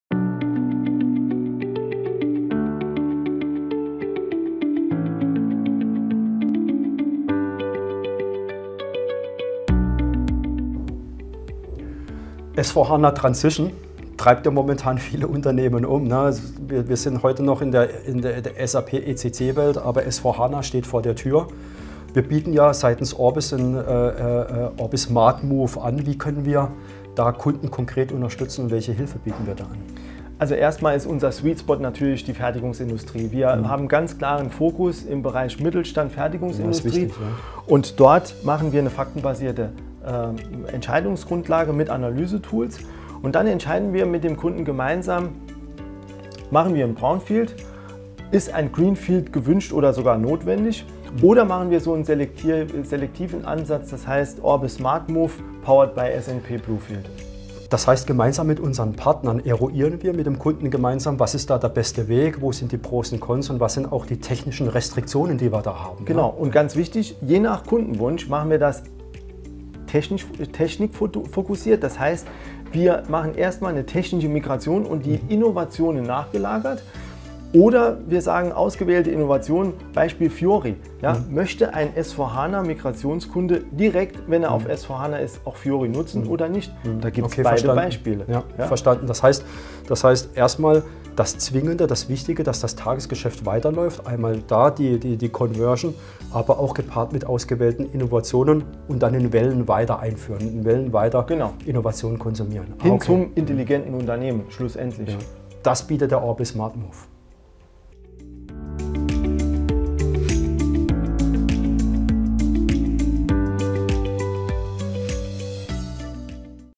ORBits Thema: ORBIS Smart Move
Wie unterstützen wir  Unternehmen aus der Fertigungsindustrie konkret auf Ihrem Weg zu SAP S/4HANA mit ORBIS Smart Move?